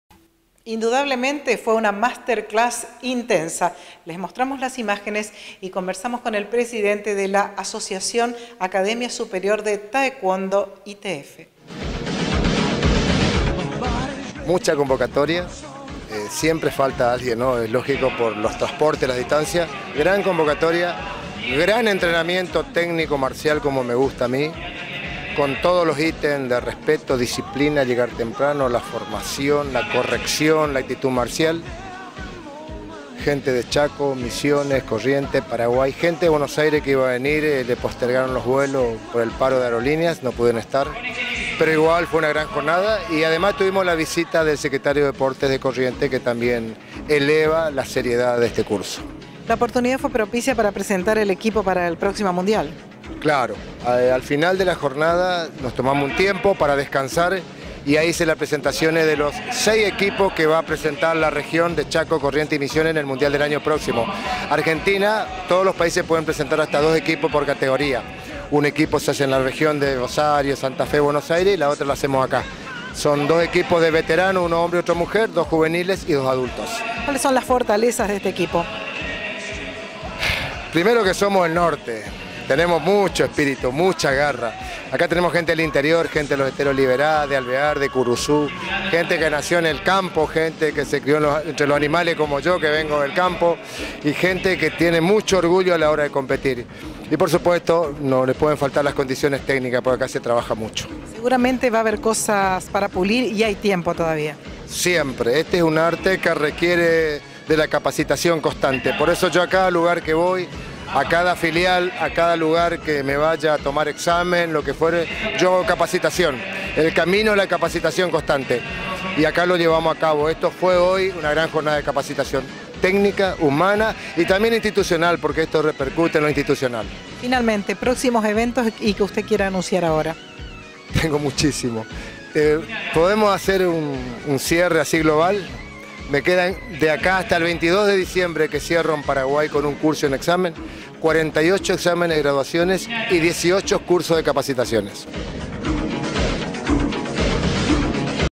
Asociación Academia Superior de Taekwondo I T F
Se realizó en nuestra Capital, una MASTER CLASS con Delegaciones de diferentes partes de la Argentina y Paraguay